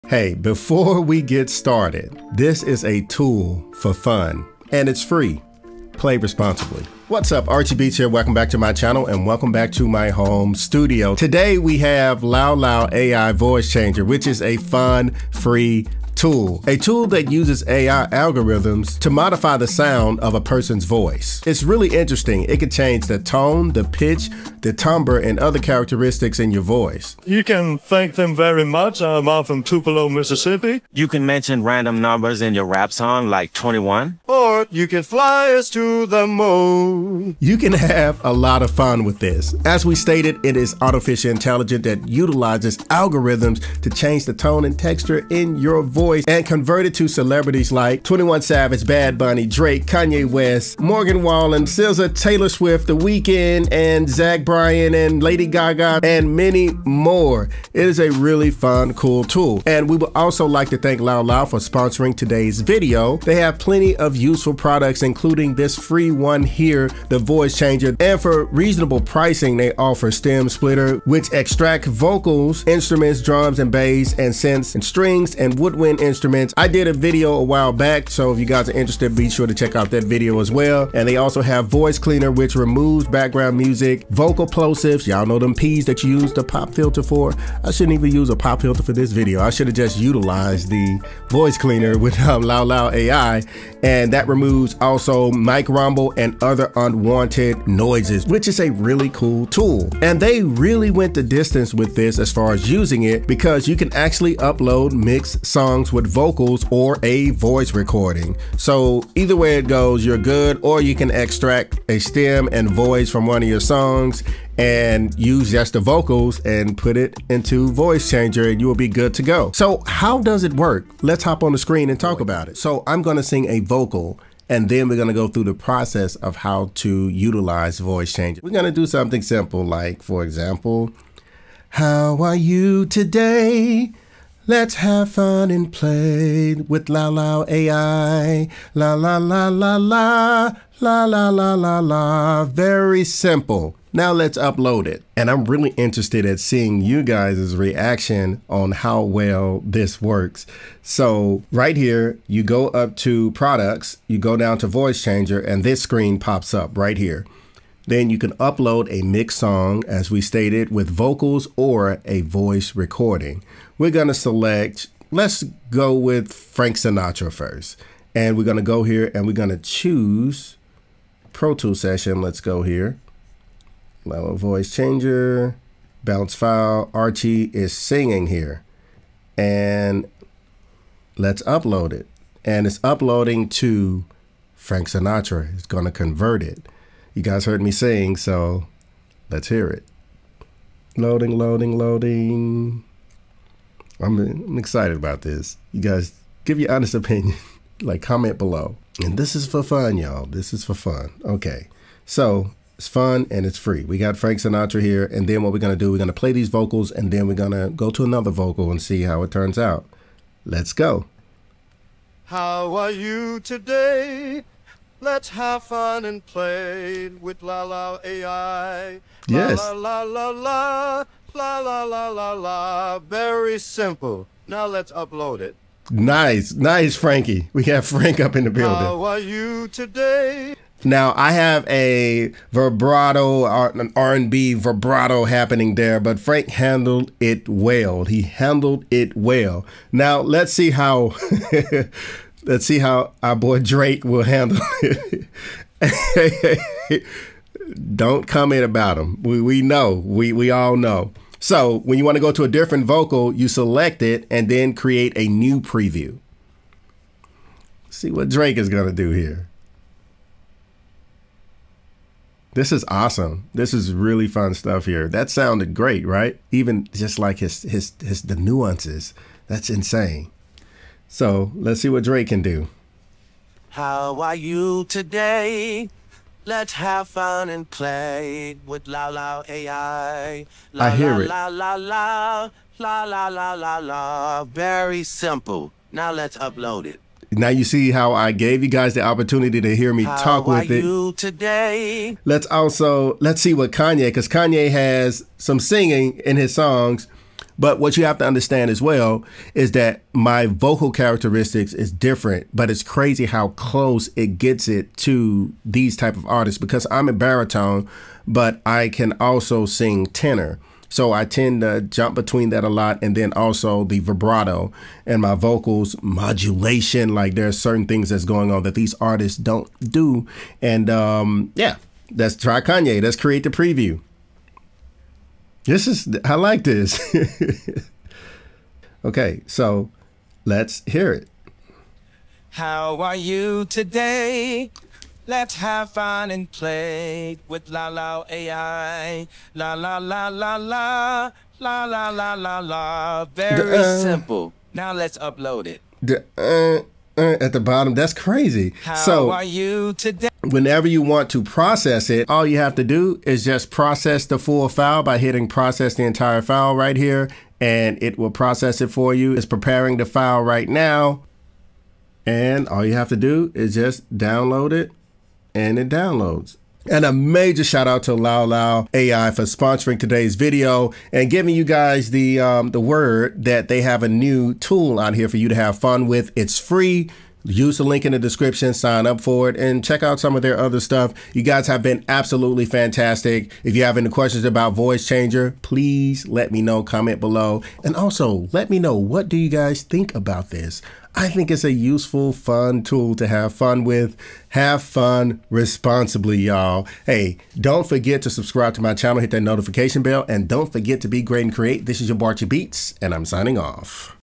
FREE AI PLUGIN - LALAL.AI Voice Changer
In today's video, we dive into the FREE LALAL Voice changer! Get it for free, play around with it and let me know your thoughts!

LALAL Voice Changer
AI Vocal plugin
Music Production
Ai Music Production tool